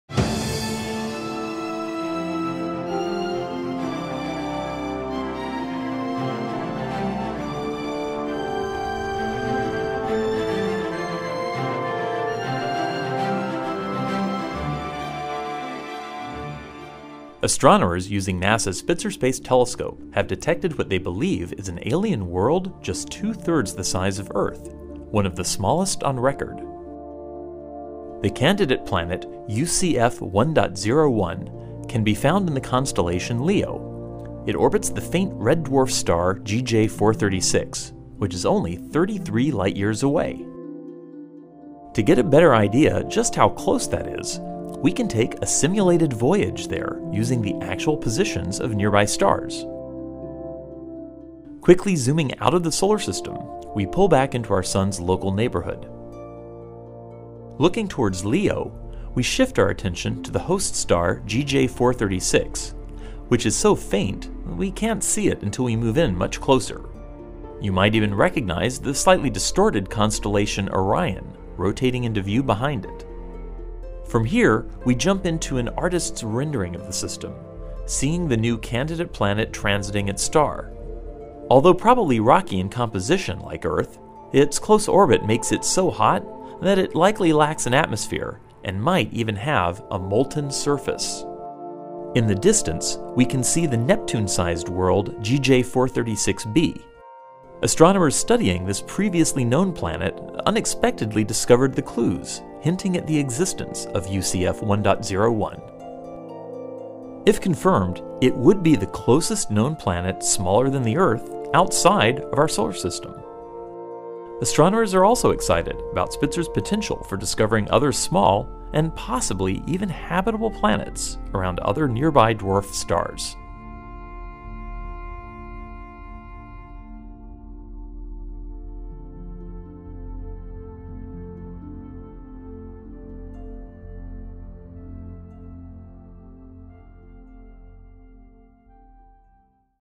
Spitzer: Possible Nearby Exoplanet Smaller than Earth
'Astronomers using NASA's Spitzer Space Telescope have detected what they believe is an alien world just two-thirds the size of Earth - one of the smallest on record!'